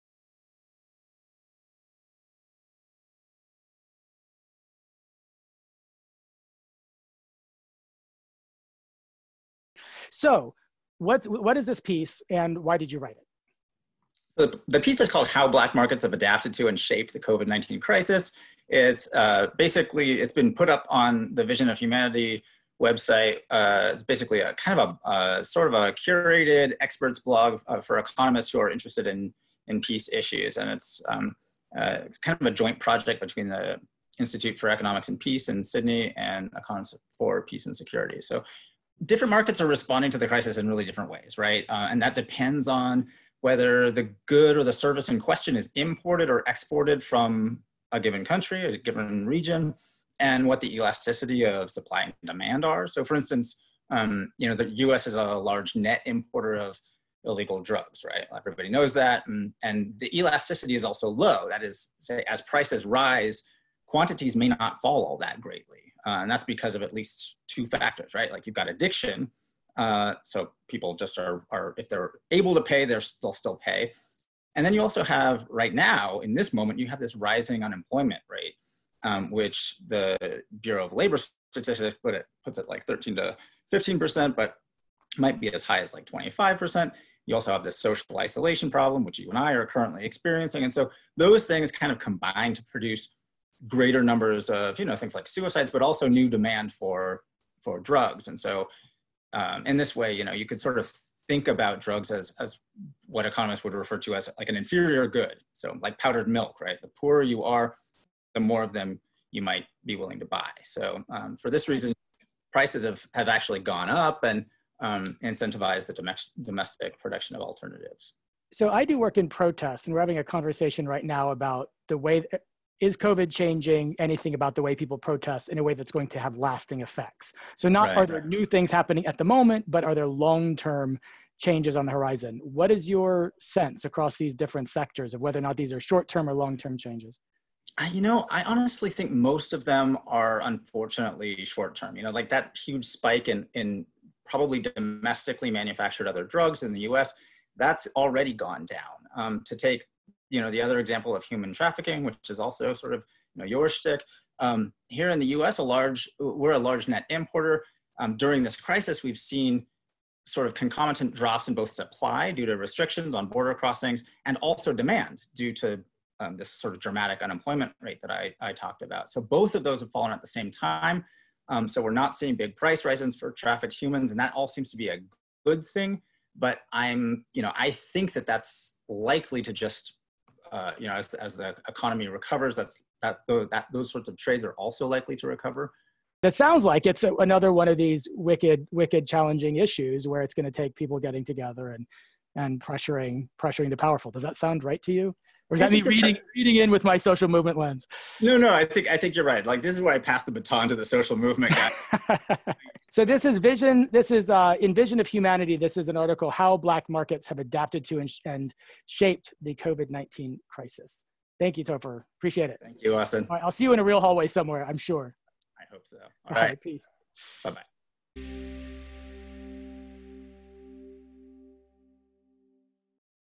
Kroc School Professor Topher McDougal Interviewed about Black Markets and the Covid-19 Crisis
Topher McDougal is Associate Professor at the Kroc School of Peace Studies at the University of San Diego. Following his article published in Vision of Humanity called "How black markets have adapted to - and shaped - the COVID-19 crisis", Kroc School Associate Professor Austin Choi-Fitzpatrick interviewed him to learn more.